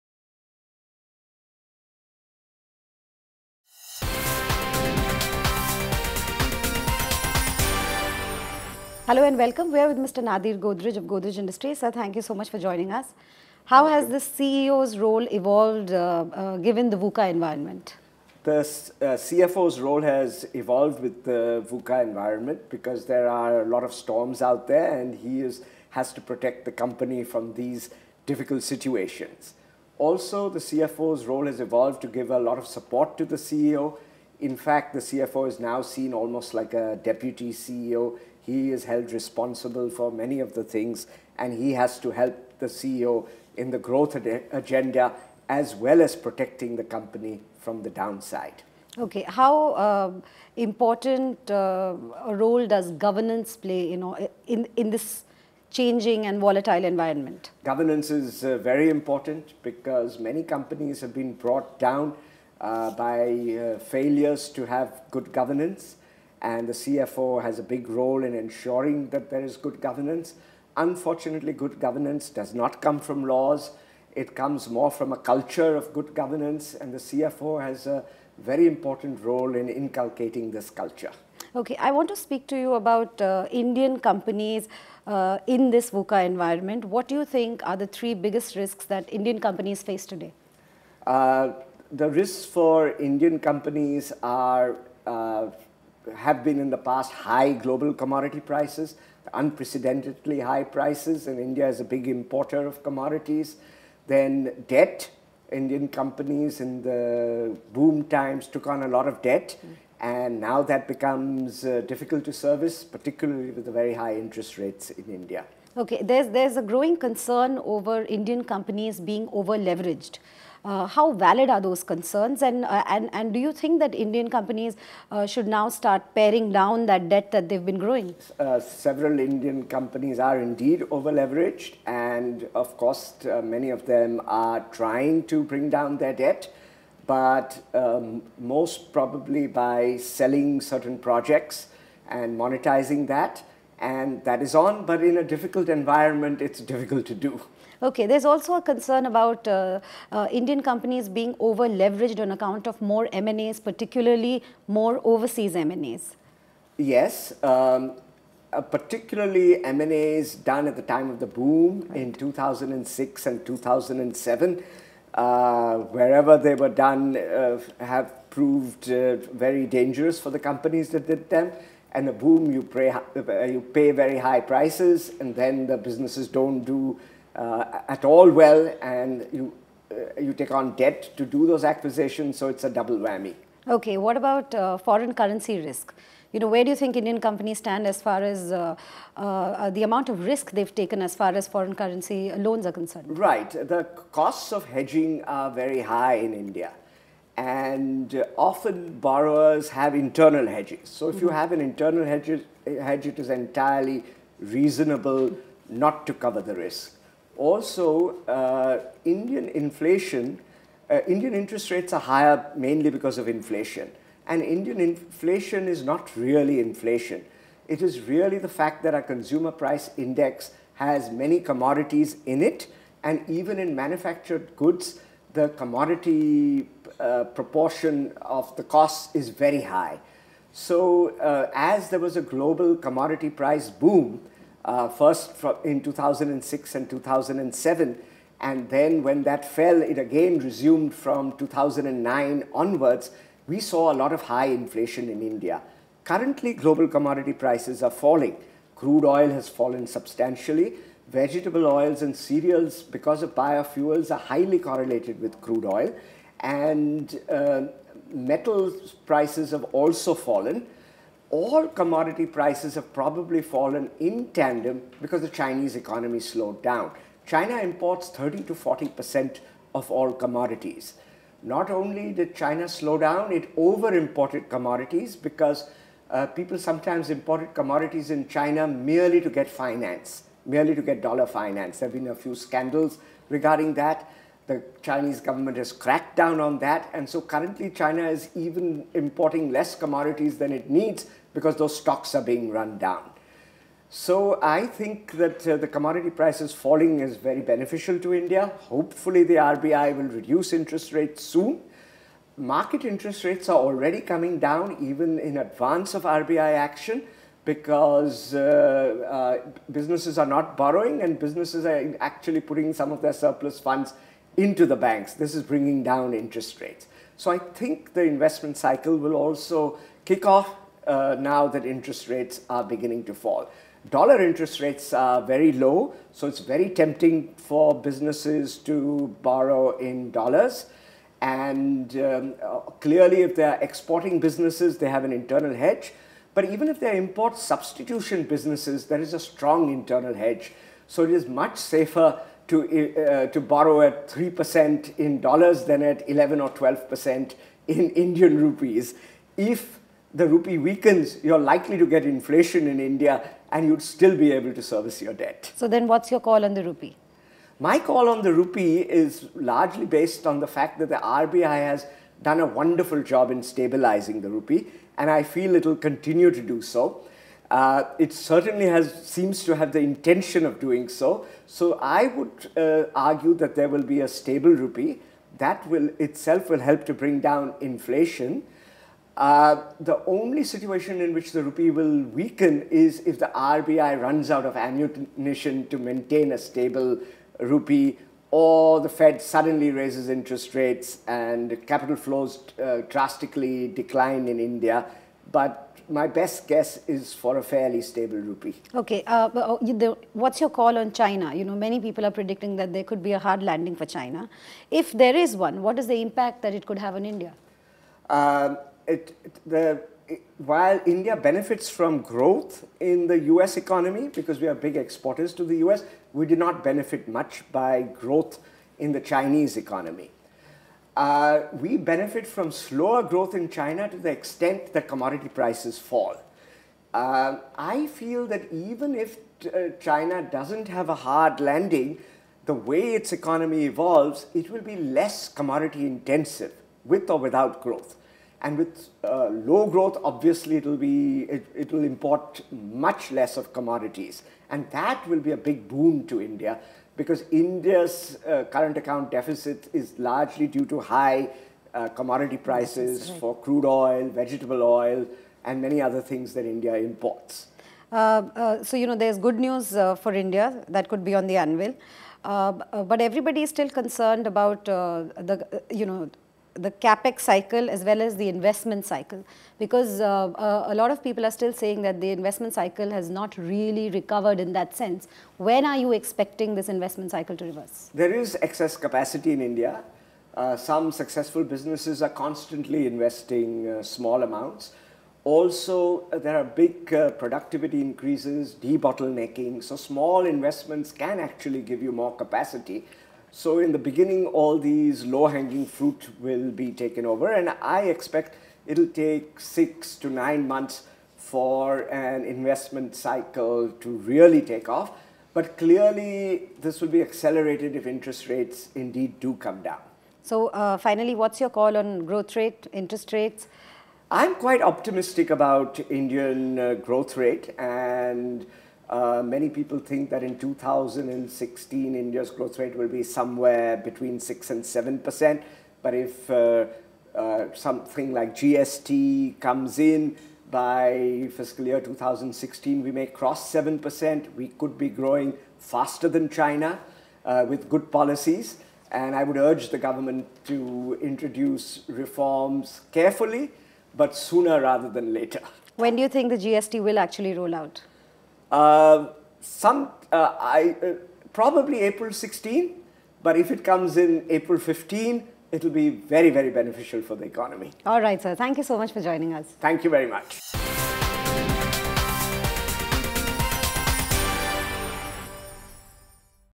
Nadir B Godrej, Chairman, Godrej Agrovet & MD, Godrej Industries
Nadir B Godrej, Chairman, Godrej Agrovet & MD, Godrej Industries in conversation with Payal Bhattar, PING Network talks about CEO's rle getting evolved in the VUCA environment.